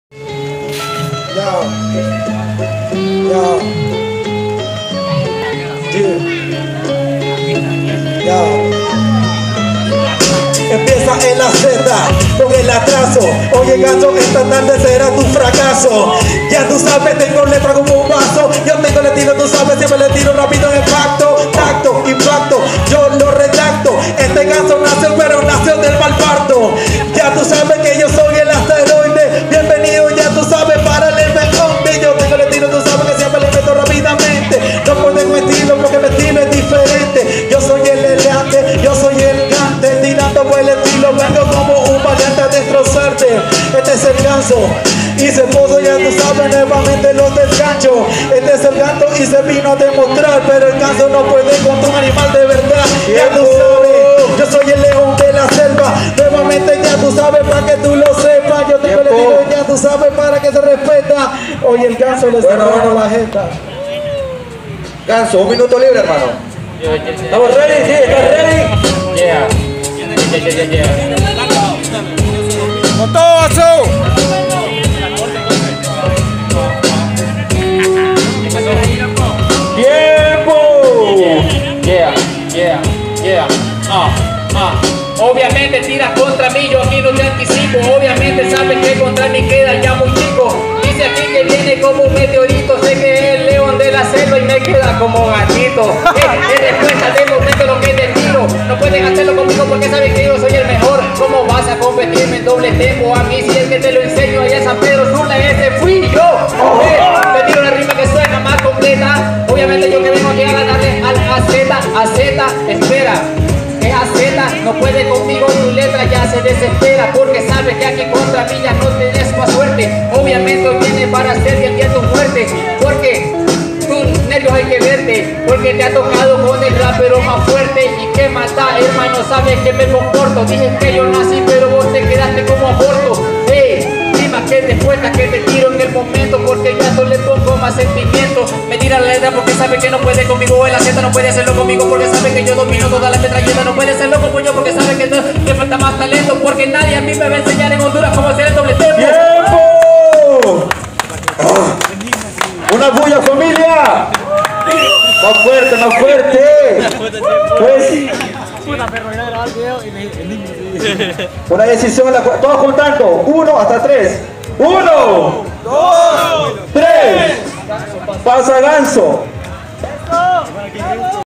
Ganzo vs AZ || Sin Limites 2016
Torneo: Sin Limites
Ciudad: Choloma
Ronda: 4tos (Primera)
Formato: Muerte Súbita
Jurado: K-tra
Fecha: 23 de julio del 2016